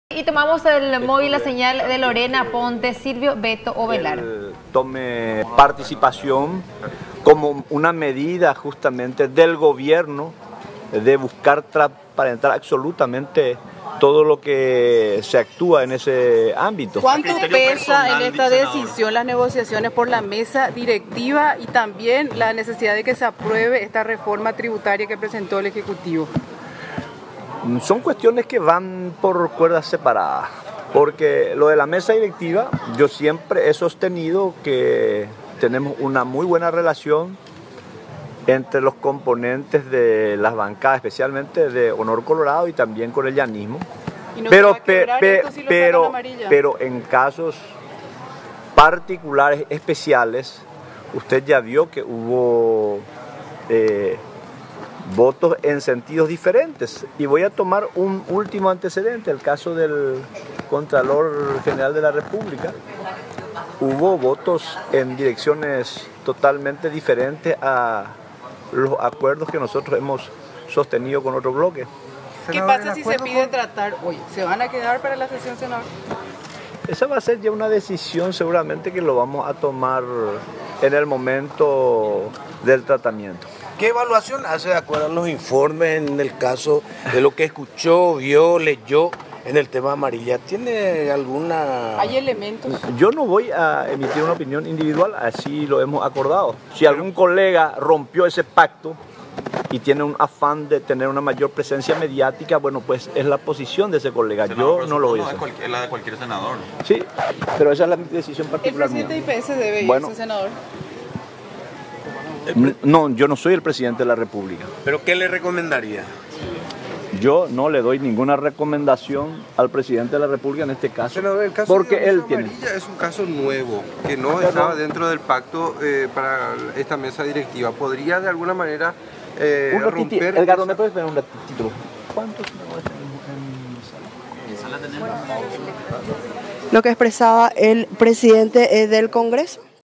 Silvio Ovelar: "No voy a emitir una opinión individual"
El presidente del Congreso, Silvio Ovelar, habló sobre la reforma tributaria y el caso del supuesto tráfico de influencia de Dionisio Amarilla.